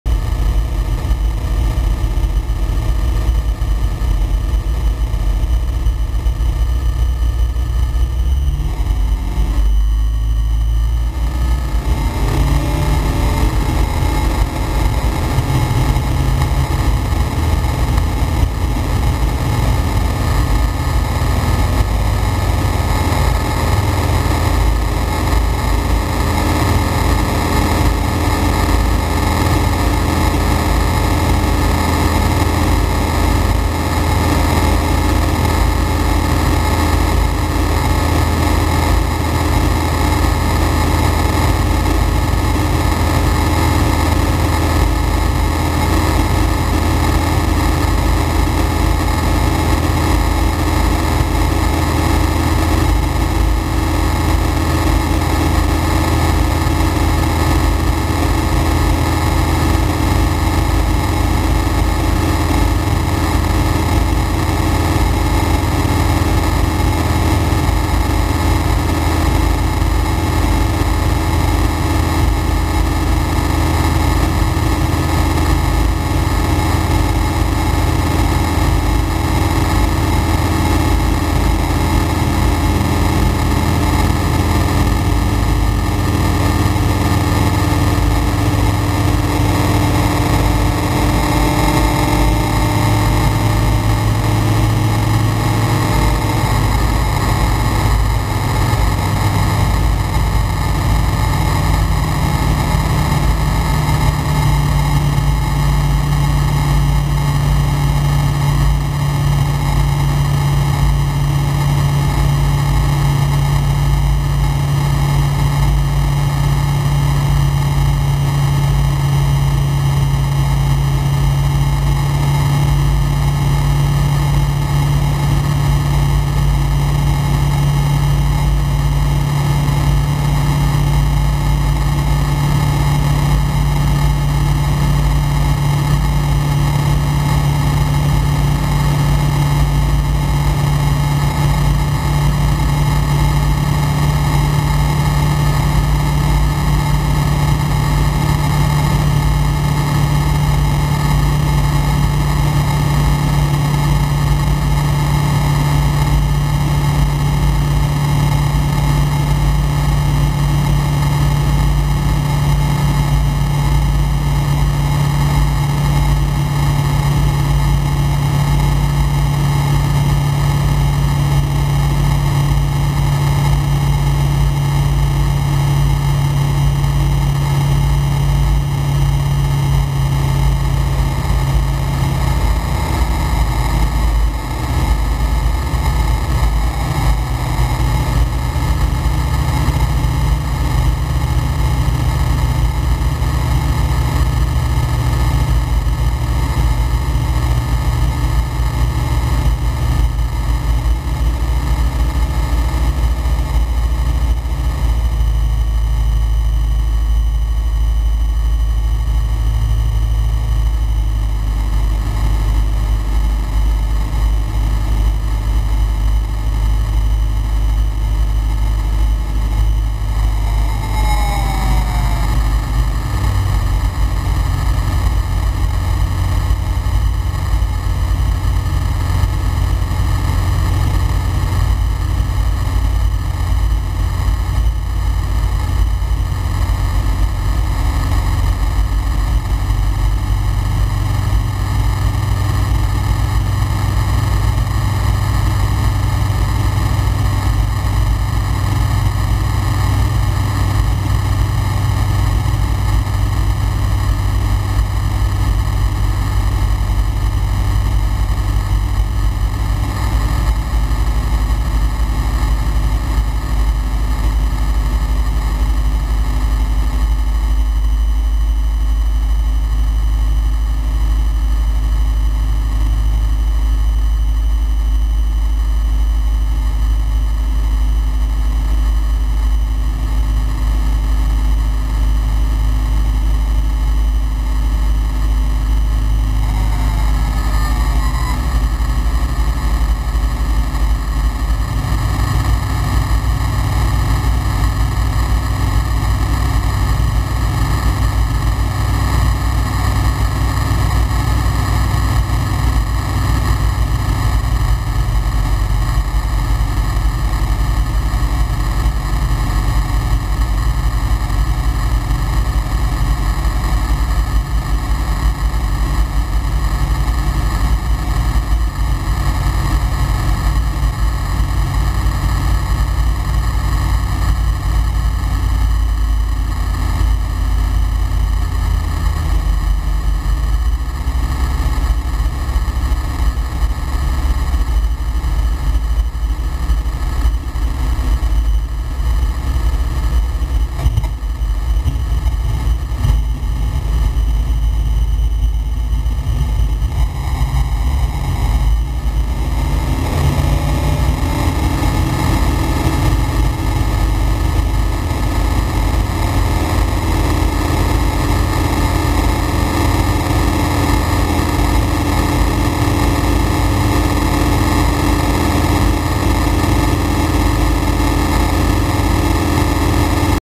My first solo flight with CTLS
Můj první solo let s CTLS nad letištěm v Příbrami LKPM